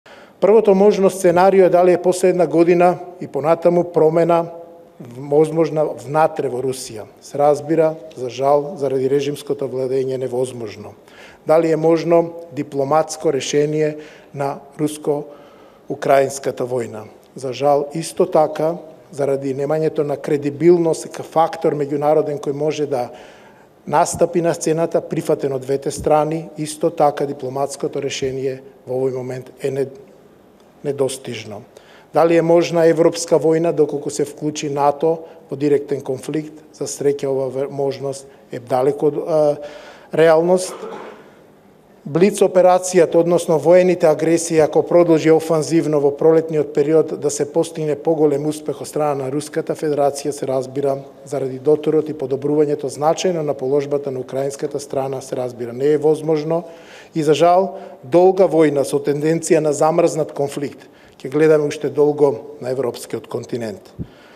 Ѓуровски: Долга војна со тенденција на замрзнат конфликт ќе гледаме долго на европскиот континент
Во контекст на руската агресија врз Украина, проф. д-р Марјан Ѓуровски, ги потенцираше можните сценарија за иходот на војната, со акцент на многу подбрата положба на Украина во моментот, на сметка на Русија.
Какви сценарија ни престојат:
-Нова длабока воена офанзива;
-Долга војна со тенденција на замрзнат конфликт;
-Европска војна –Русија со НАТО;
-Дипломатско решение;
-Промени во Русија од внатре.

Камера: Атанас Петровски/ Александар Ризински
Реализација и монтажа: Ариан Мехмети